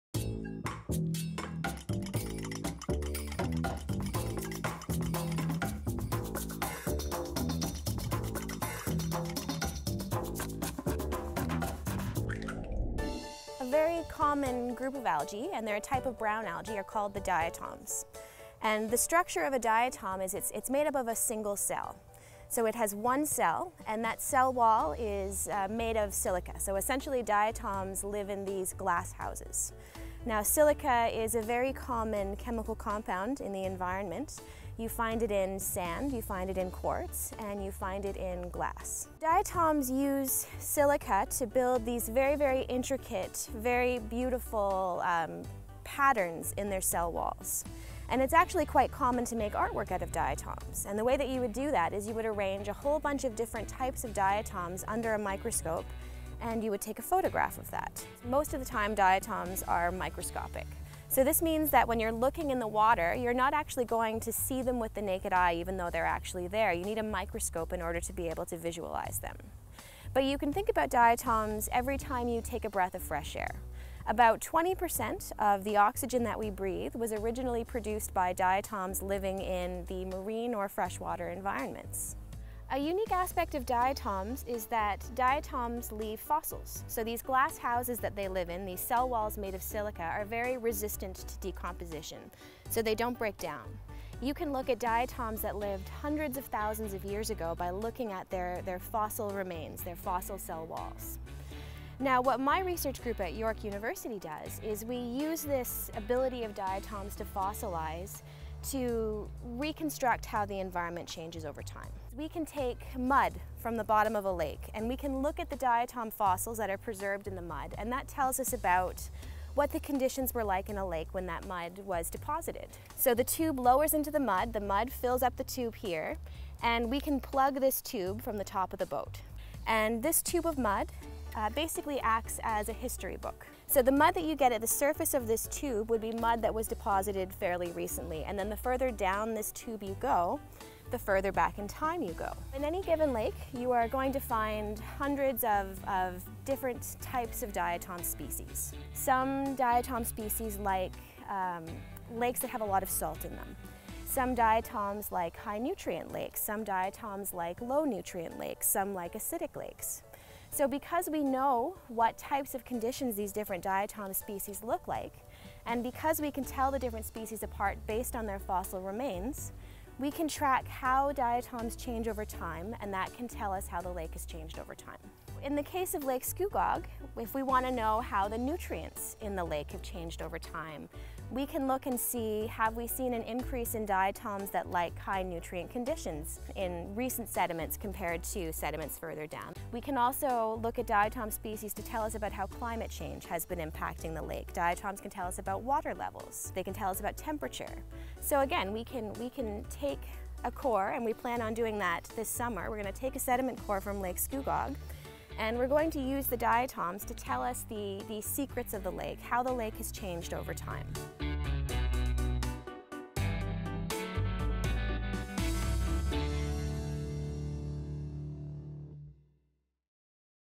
Microscopic Algae: Diatoms
Dr. Jenny Korosi is an assistant professor in the Department of Geology at York University.

In this video, she explains what diatoms are and how their fossils can be used to piece together the history of Lake Scugog.